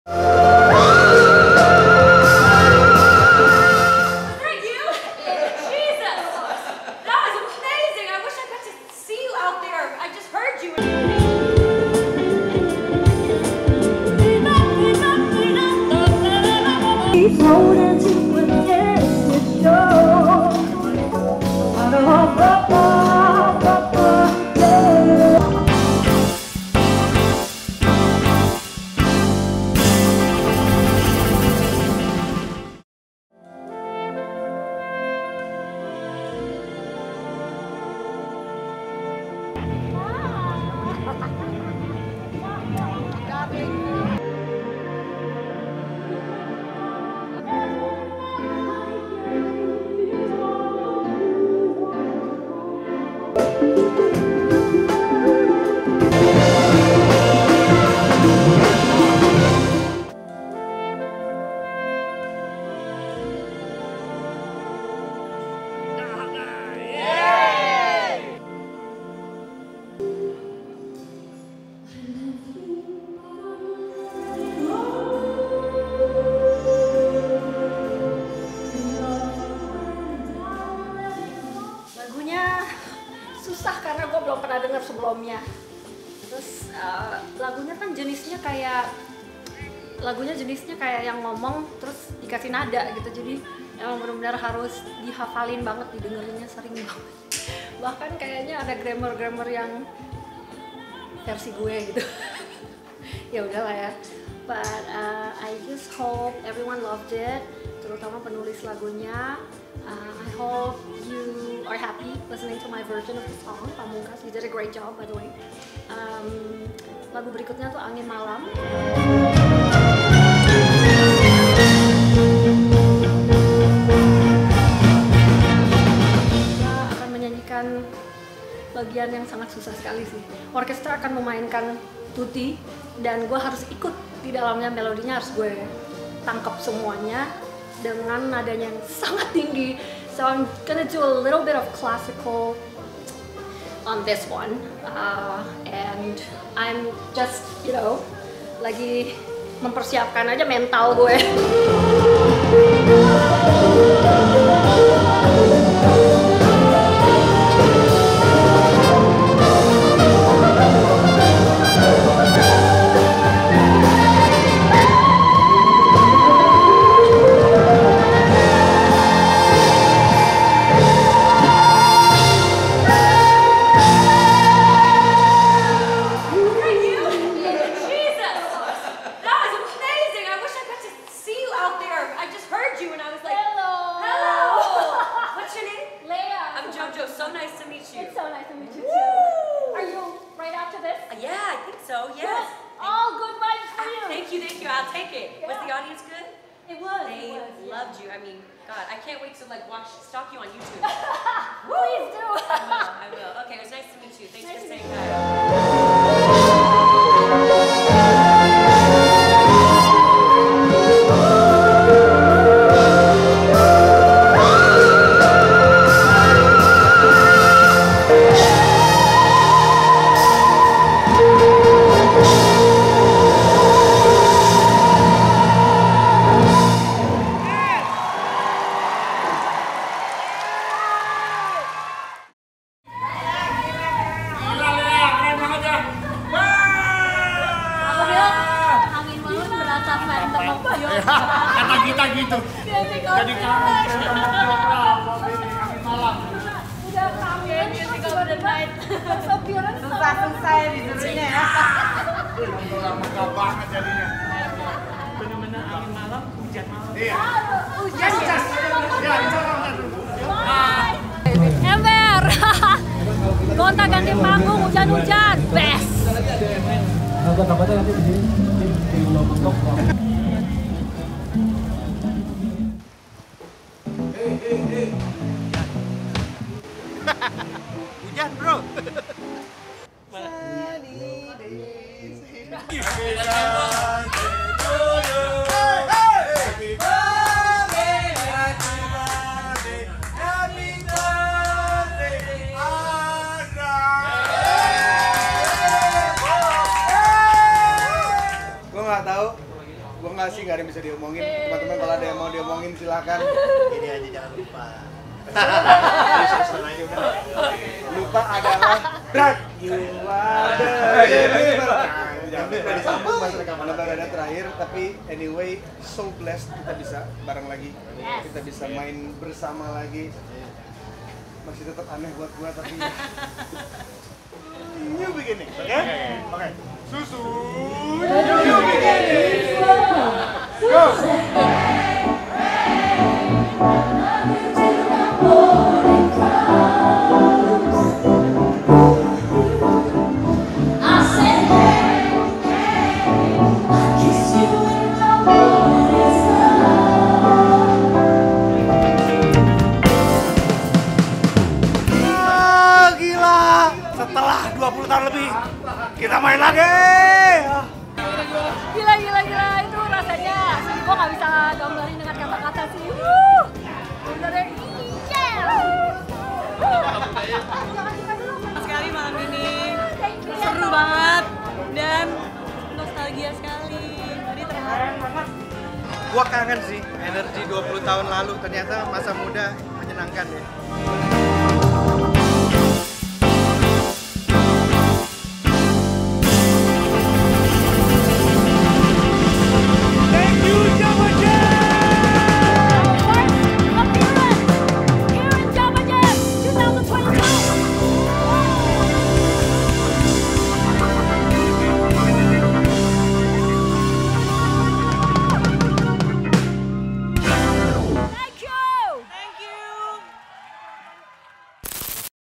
MY FIRST JAVA JAZZ PERFORMANCE SINCE PANDEMIC - Lea Simanjuntak
Lea Simanjuntak at Java Jazz Festival 2022. Performing with Erwin Gutawa at Indoor Stage and SUSU band at Outdoor Stage.

Video by bowohariyo